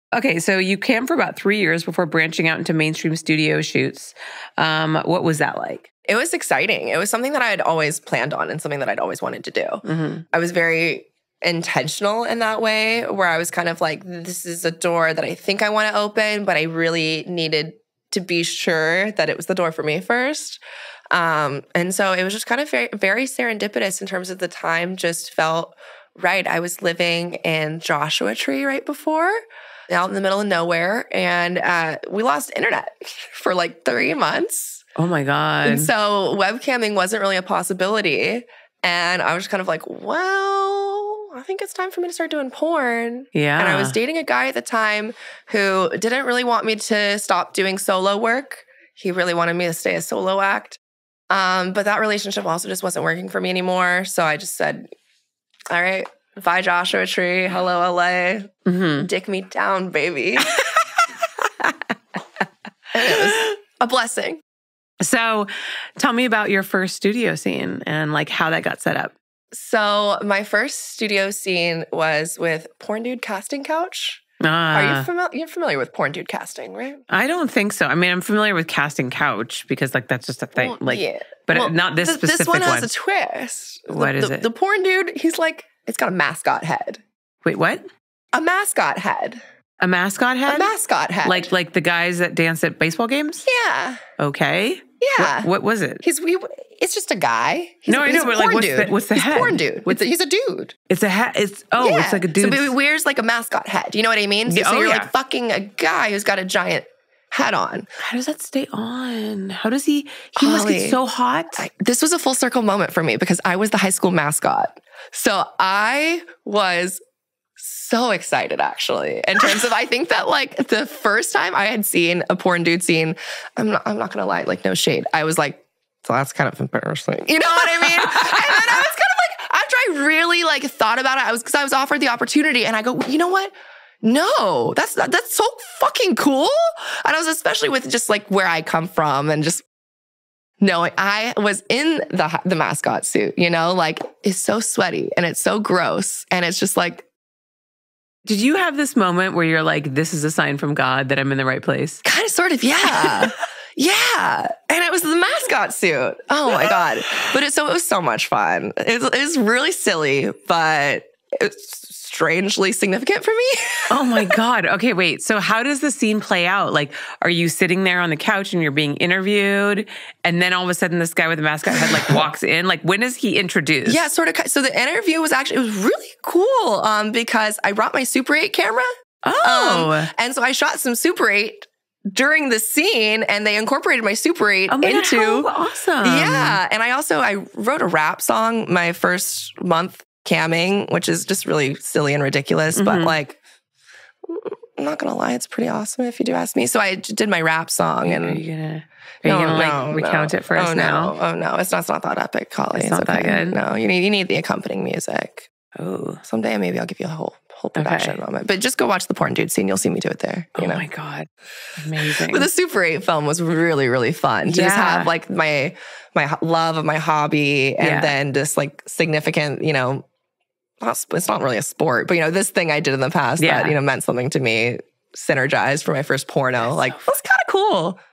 Millie Morgan’s First Scene Was With a Team Mascot 🐻‍❄️
Millie Morgan’s first porno was a full-circle moment getting HOT and STEAMY 🔥with The Porn Dude. Millie f***ed a mascot for her first XXX scene!